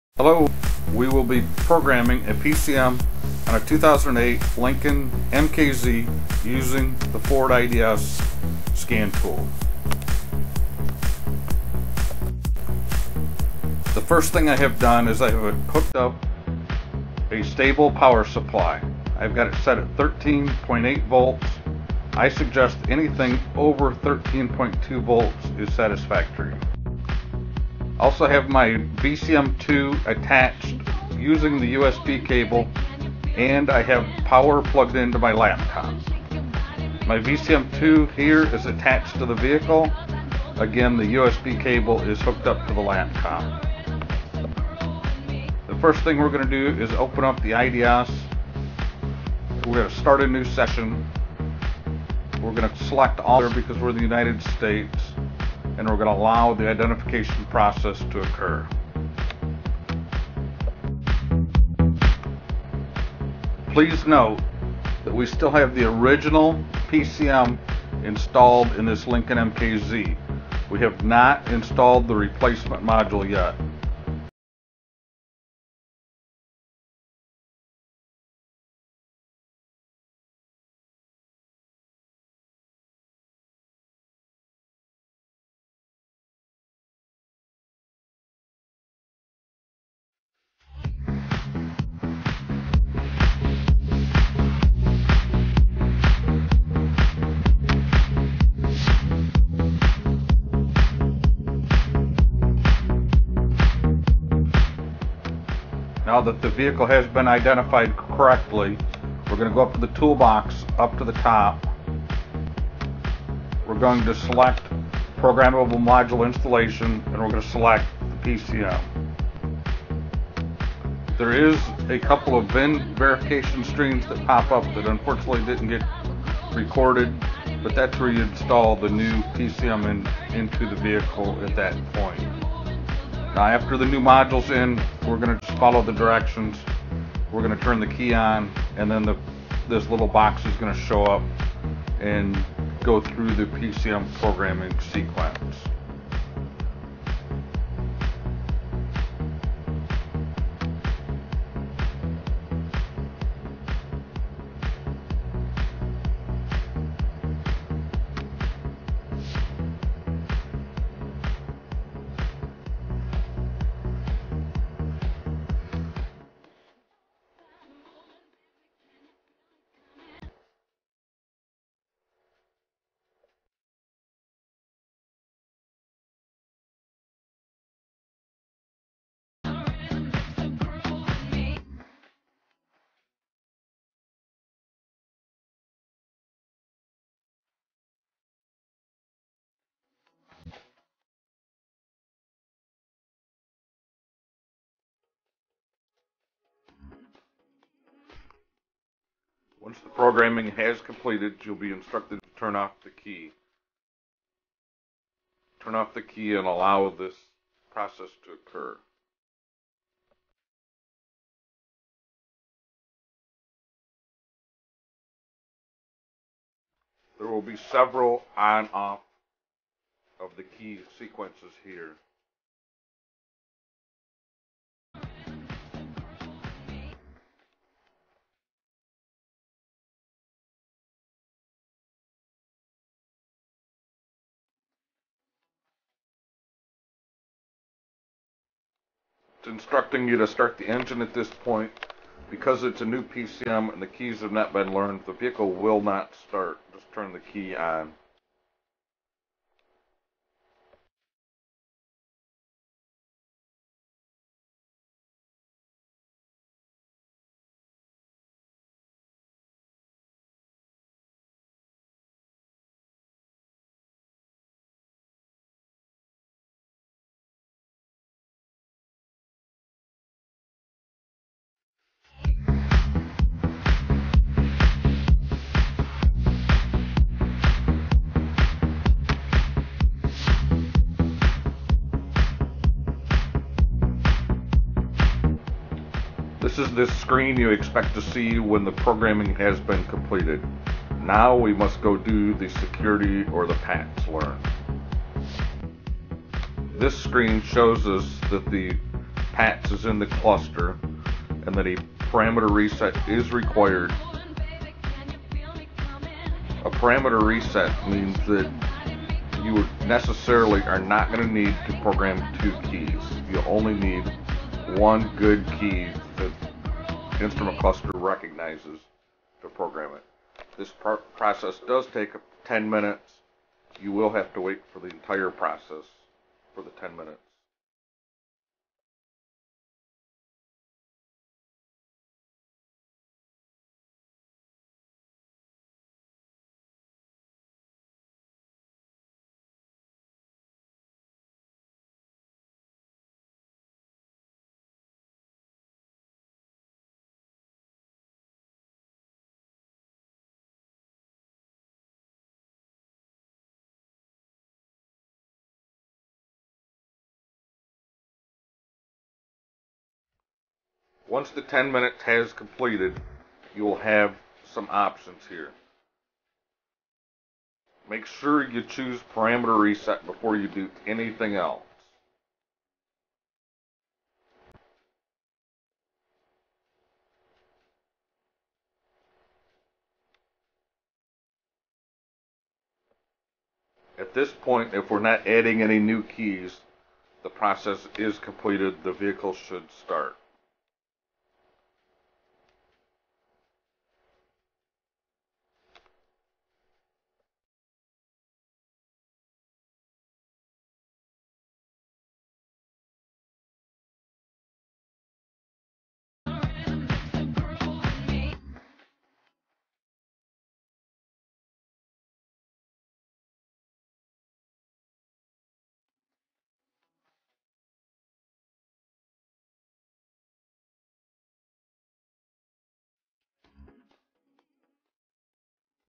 Lincoln MKZ PCM
Program new PCM Lincoln MKZ Using Ford IDS Want a ToolHutUSA sticker send $5 USD specify it is for a sticker. Outside US please add some extra for additional postage.